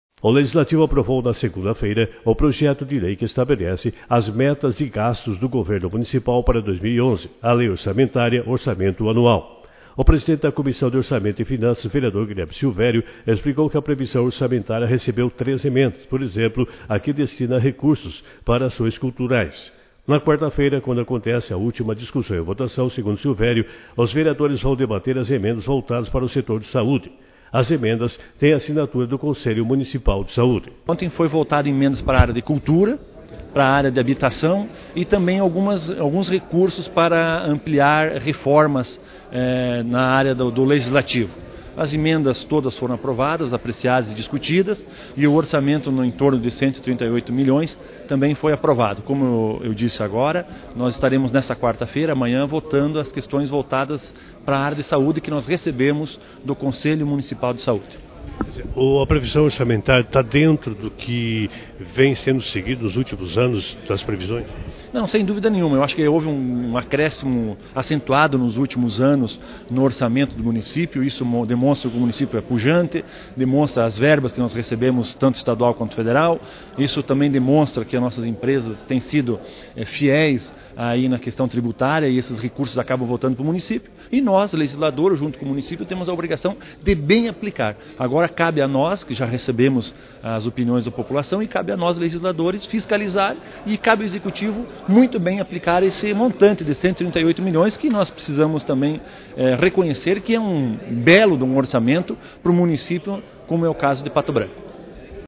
Vereadores aprovaram orçamento de R$ 138 milhões, mas com emendas
[07/12/2010] O Legislativo  aprovou na segunda-feira, dia 6 de dezembro, o Projeto de Lei n.º 182/10 que estabelece as metas de gastos do governo municipal para 2011, a Lei de Orçamentária- Orçamento Anual (LOA).